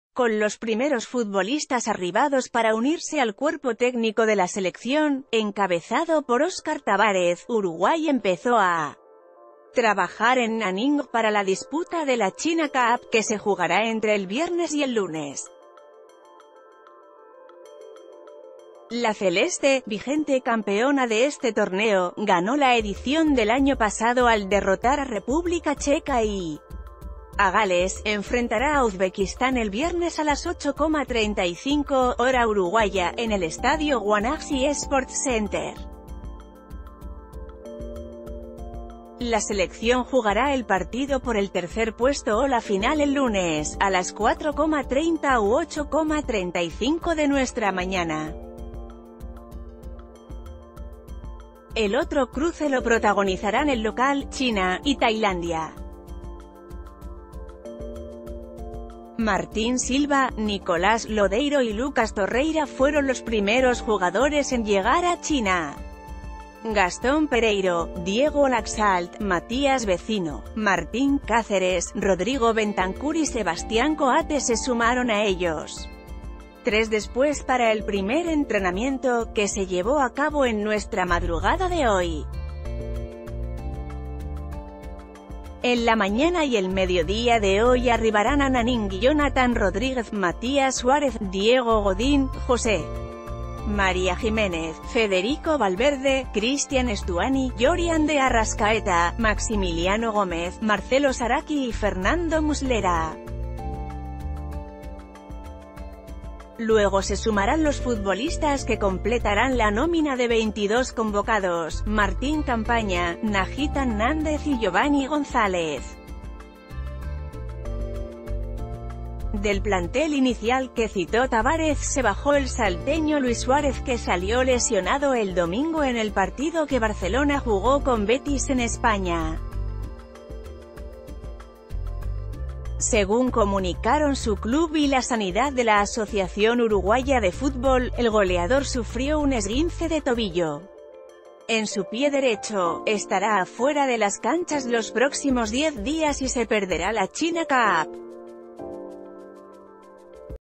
La selección se prepara en Nanning para la China Cup
Con los primeros futbolistas arribados para unirse al cuerpo técnico de la selección, encabezado por Óscar Tabárez, Uruguay empezó a trabajar en Nanning para la disputa de la China Cup, que se jugará entre el viernes y el lunes. La celeste, vigente campeona de este torneo (ganó la edición del año pa...